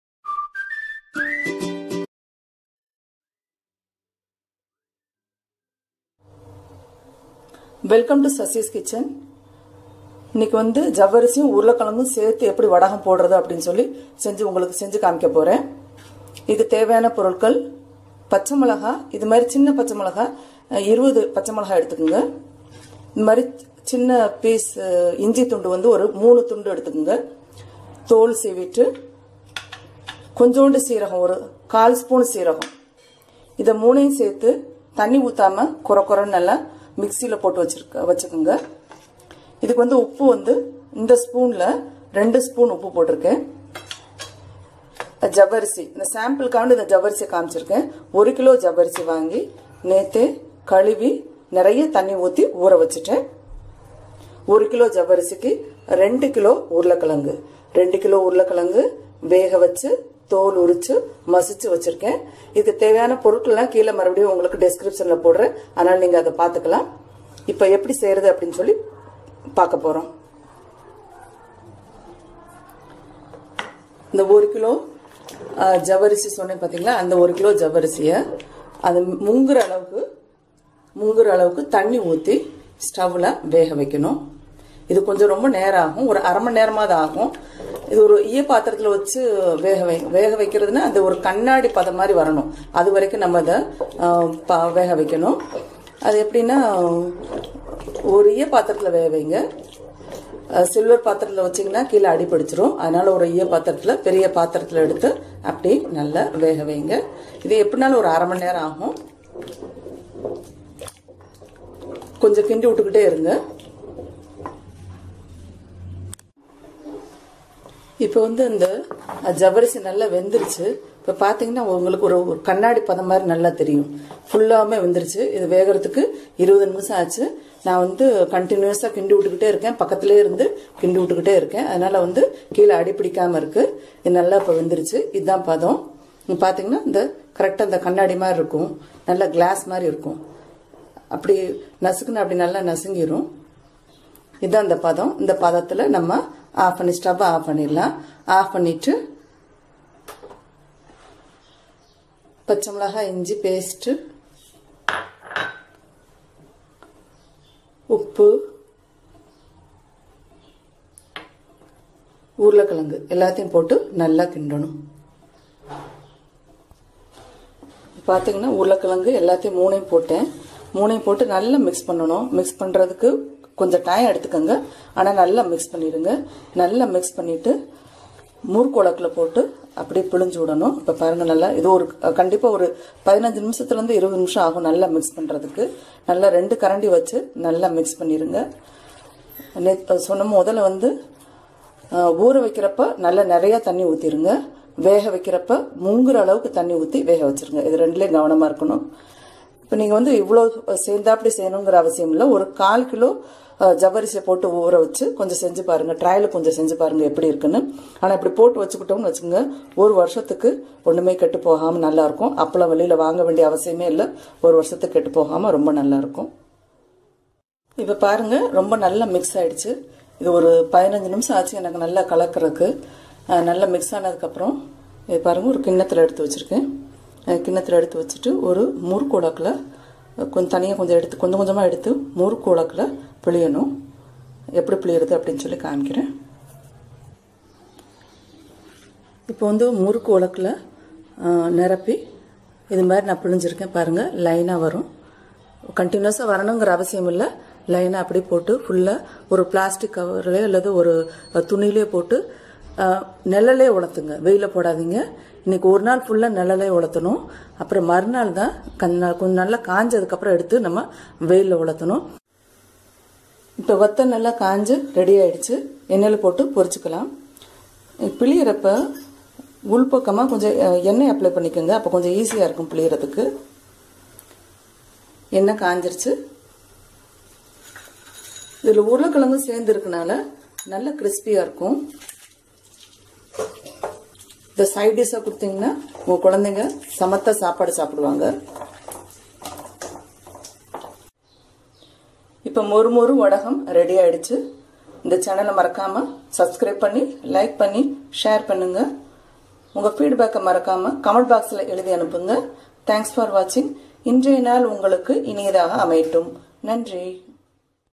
Potato with rice(ஜவ்வரிசி) finger chips(வடகம்)/ Crispy vadagam stored for a year/Crispy finger chips
sasi's kitchen.
Ingredients.     1kg- Javvarishi
2kg    potatoes
ginger, green chillies and cumin seeds